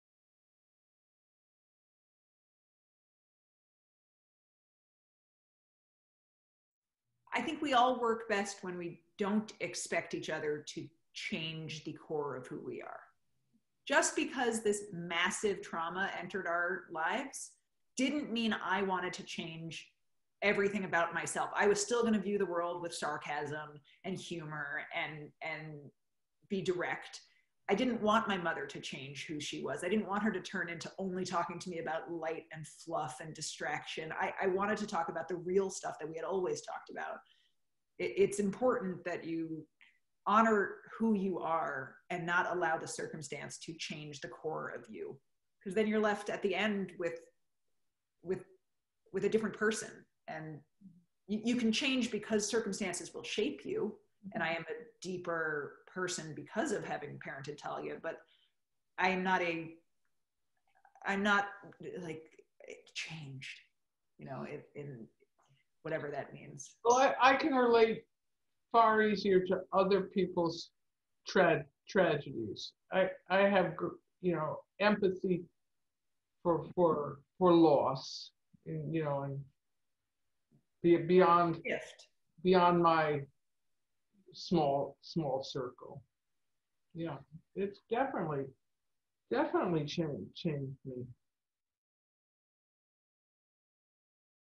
“I think we all work best when we don’t expect each other to change the core of who we are.”
Carla, mother of Nathan, Audrey, and Talia (died at 23 months from infantile Tay-Sachs) speaks frankly about she didn't want or expect their circumstances to change her. "You can change because circumstances will shape you, and I am a deeper person because of having parented Talia, but I am not CHANGED."

to hear more stories.